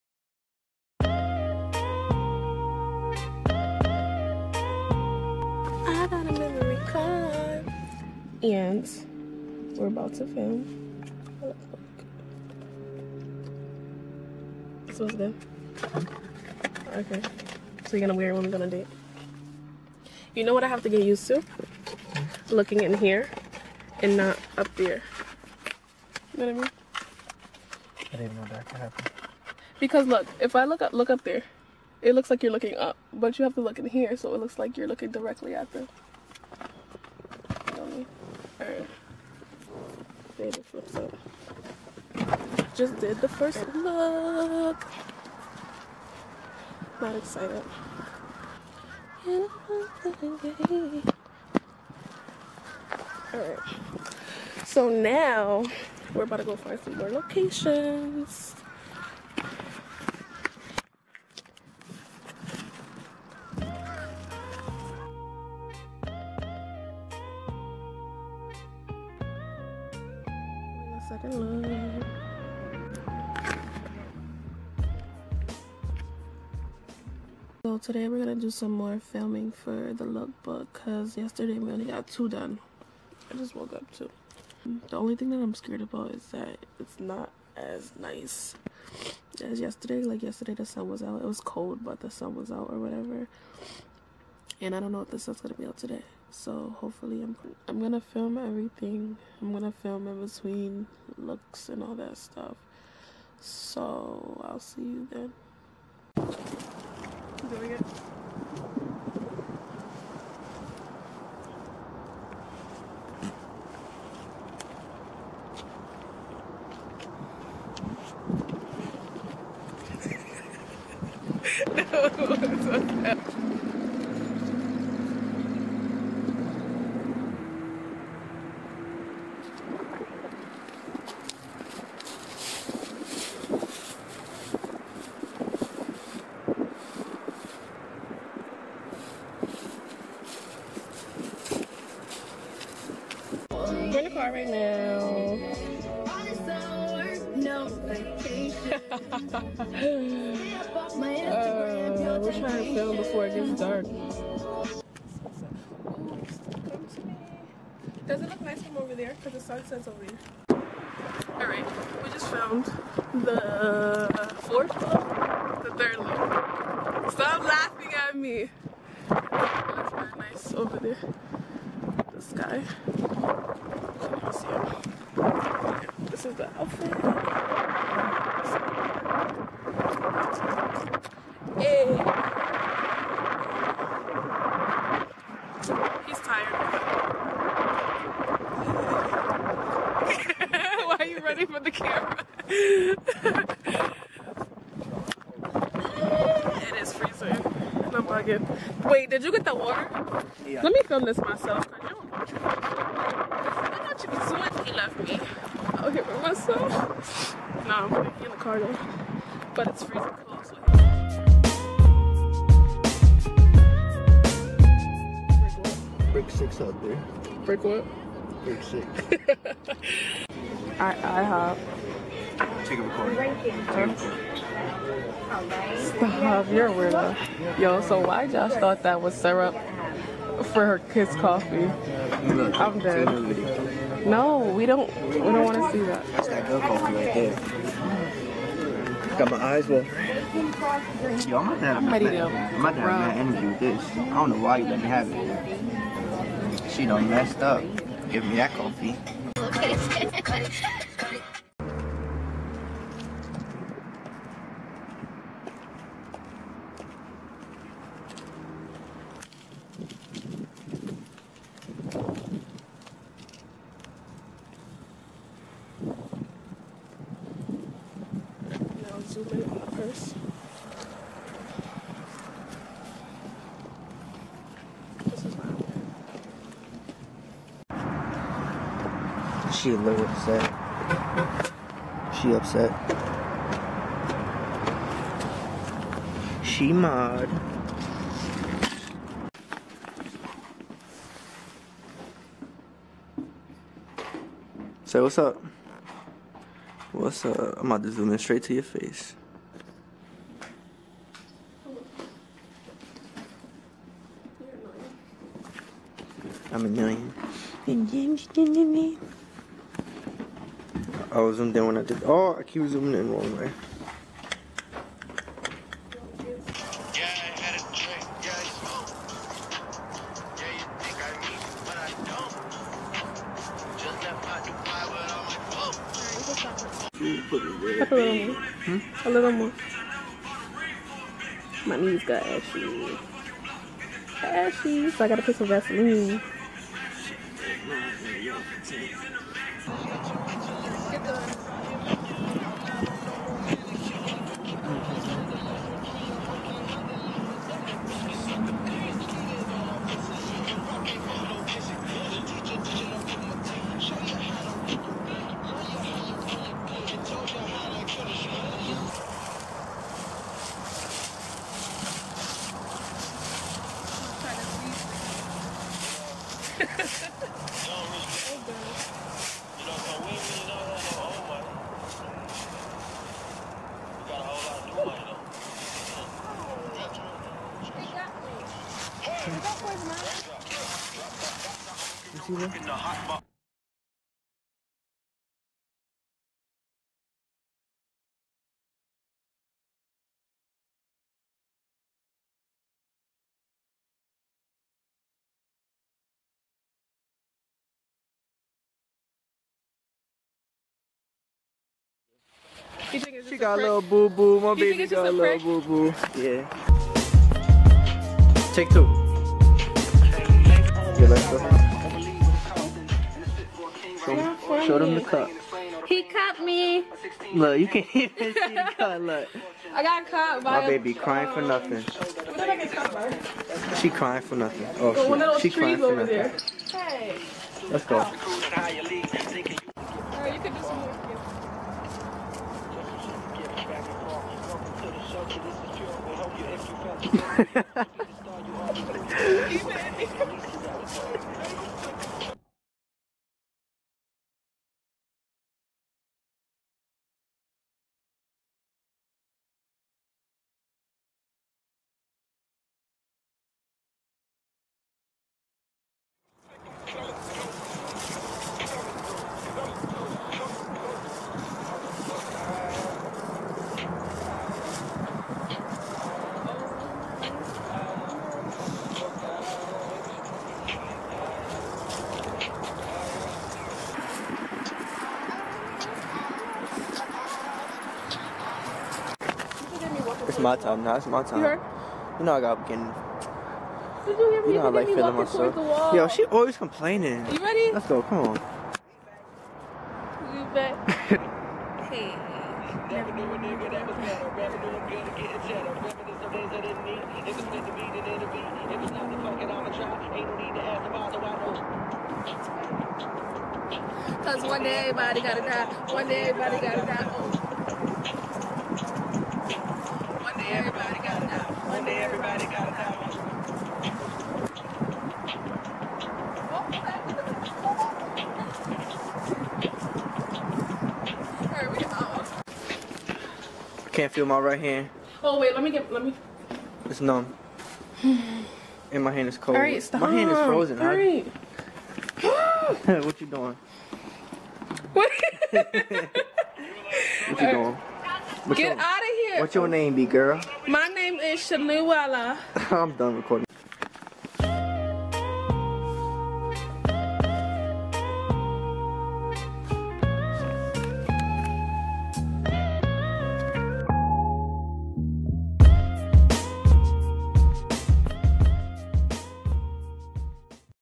He Cut Me!? Vlog #1
Vlog #1:
Hey loves! This is behind the scenes of the new year, new look lookbook.
Josh and I look for the perfect locations to film, we also have breakfast at IHOP.